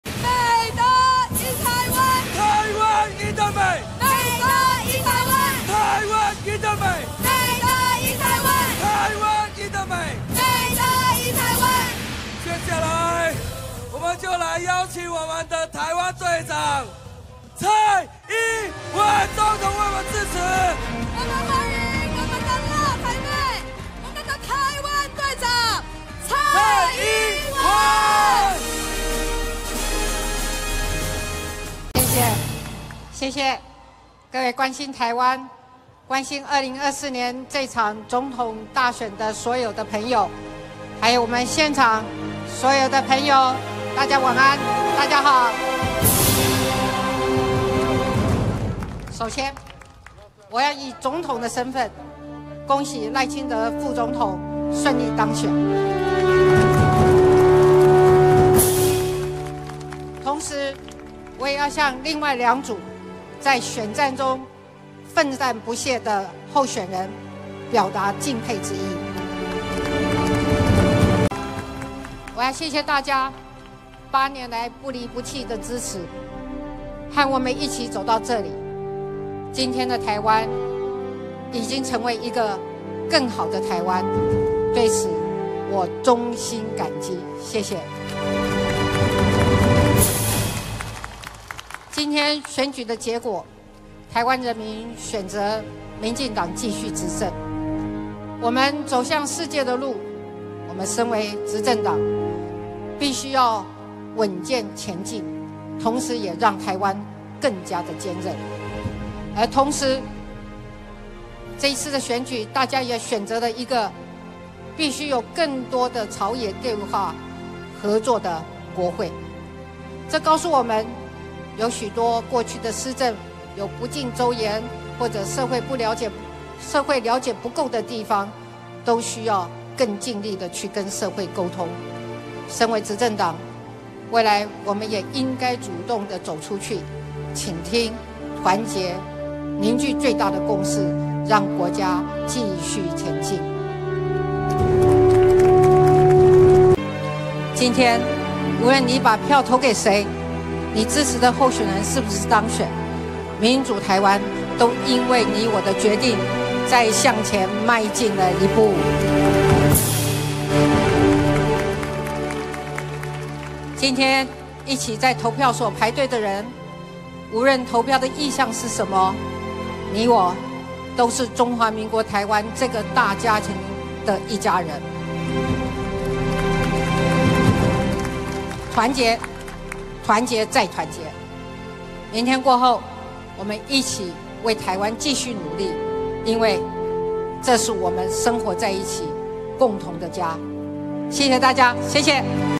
民進黨勝選之夜 蔡英文致詞：團結凝聚最大的共識｜NOWnews
2024總統、立委大選自今（13）下午4時進行開票，台南率先由第5選區的林俊憲自行宣布當選奪下一席以後，第6選區積極尋求連任的民進黨立委候選人王定宇，也在票數過半後在東區的競選總部自行宣布當選，同時呼籲民眾支持民進黨正副總統候選人，強調若民進黨正式勝出，將會是台灣憲政史上「第一次正副總統都來自台南」